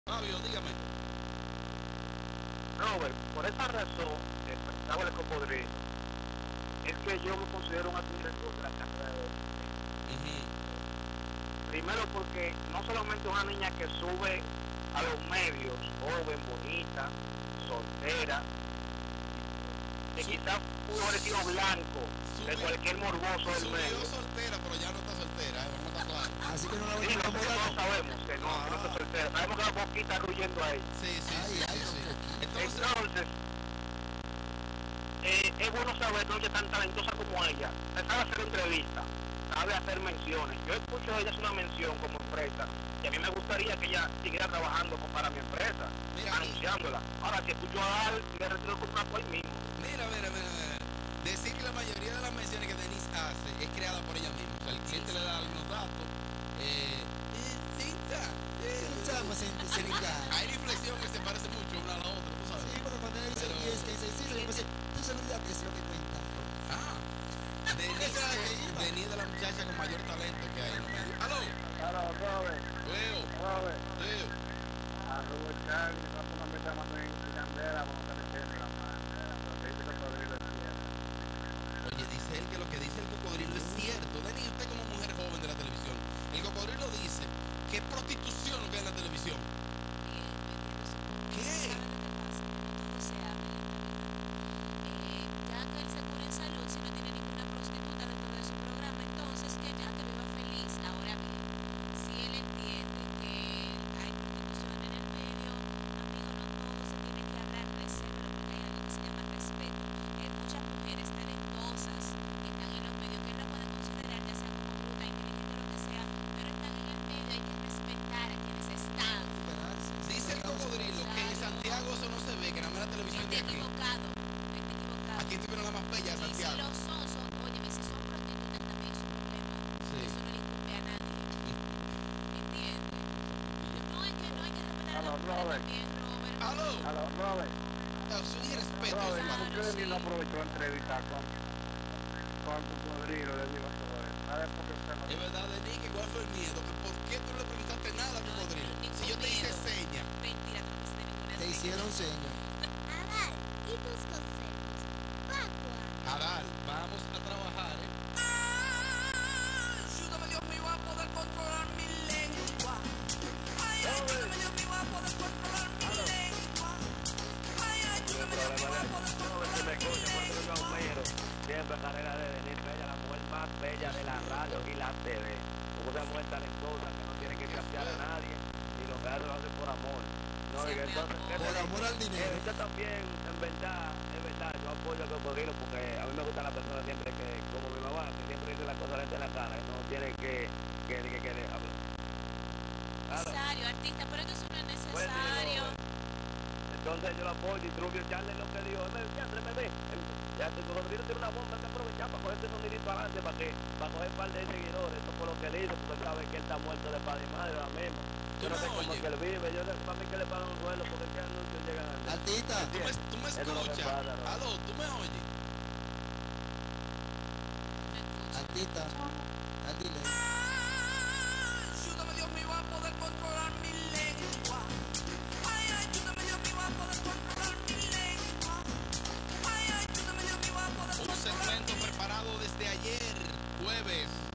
Denisse Sale en defensa de las mujeres del Medio y responde a Nelson Javier
Buscando Sonido con Robert Sánchez